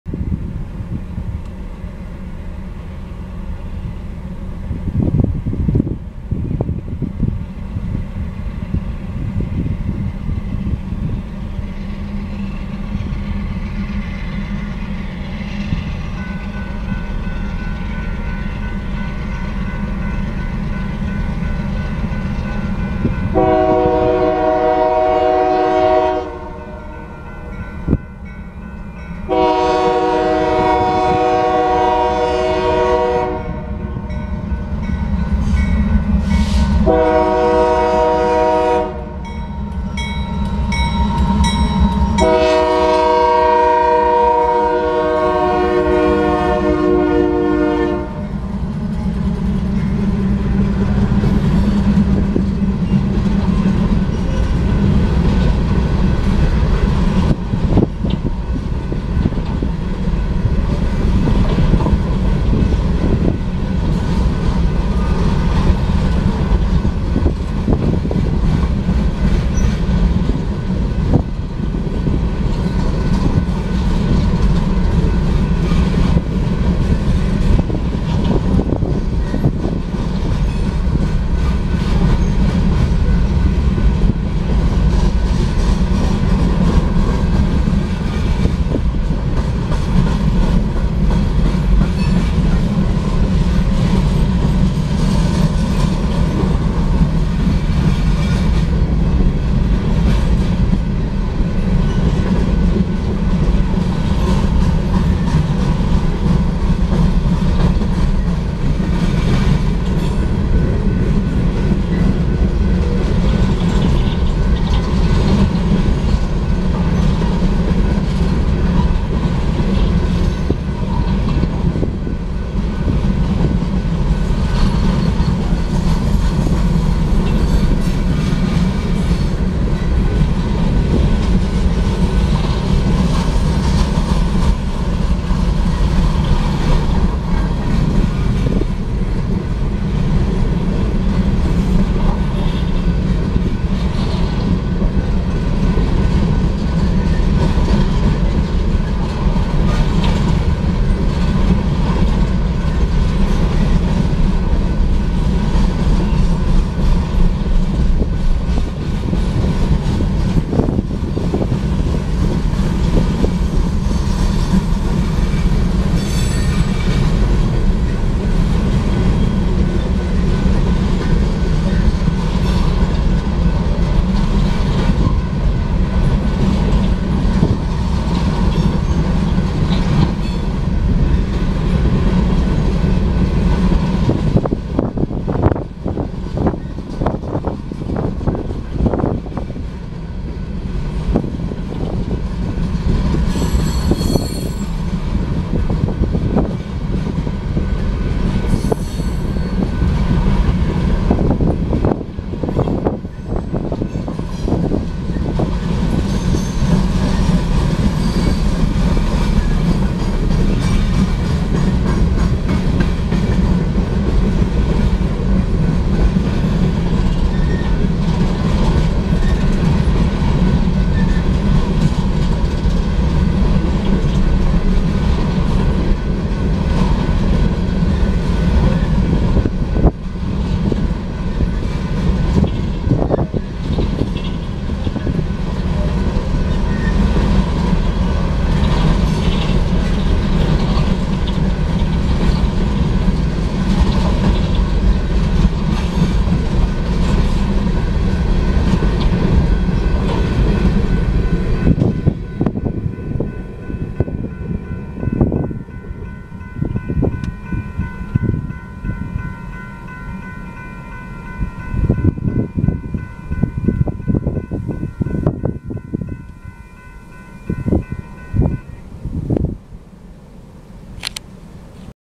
UP 6345 North O??RP Filmed At North Kasper Siding (2-8-2024)
UP 6345 (Ex-SP 296) (GE AC44CW)
North Empty Coke Train ??-Roseport, MN/North Kasper Siding Side Track to Main Track/Filming Location 227th St. East Grade Crossing-Walcott Township, MN/DOT#876382T-M.P. 296.35
UP Albert Lea Sub/2:14pm